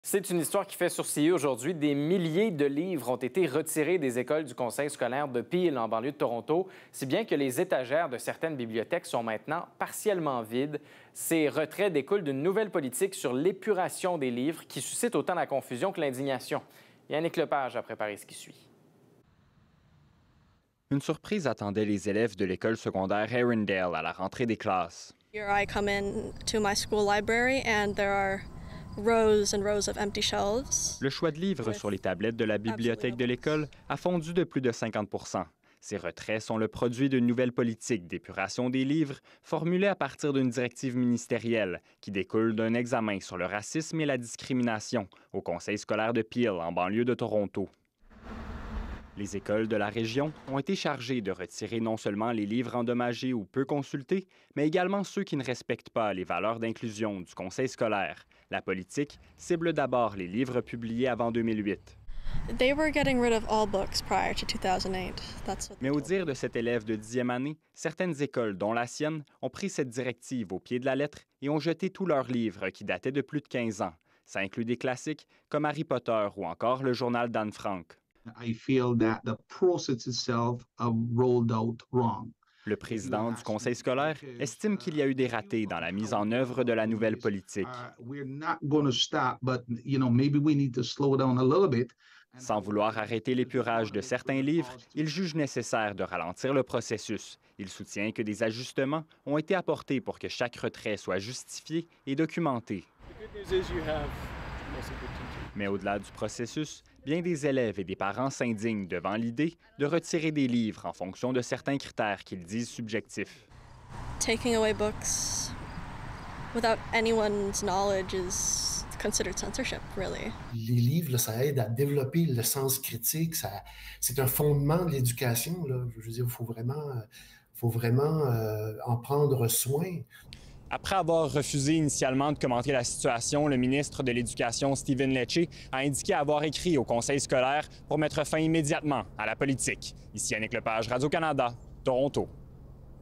Des milliers de livres retirés des bibliothèques d’écoles près de Toronto
L'élève Reina Takata raconte qu'il n'y avait plus d'exemplaires des livres de Harry Potter ou Hunger Games, par exemple, à la rentrée à la bibliothèque de son école secondaire. Face au tollé suscité par la politique d'épuration du Conseil scolaire public anglais de la région de Peel (PDSB), le ministre de l'Éducation Stephen Lecce est intervenu finalement mercredi.

Au total, des milliers de livres ont été retirés des rayons des bibliothèques du Conseil, se plaignent des parents.

La politique sur l'épuration des livres découle d'une directive ministérielle émise à la suite d'un examen sur le racisme et la discrimination au Conseil scolaire de Peel en 2020. La directive : utiliser du matériel « inclusif et adapté aux réalités culturelles ».

Le reportage de Yanick Lepage.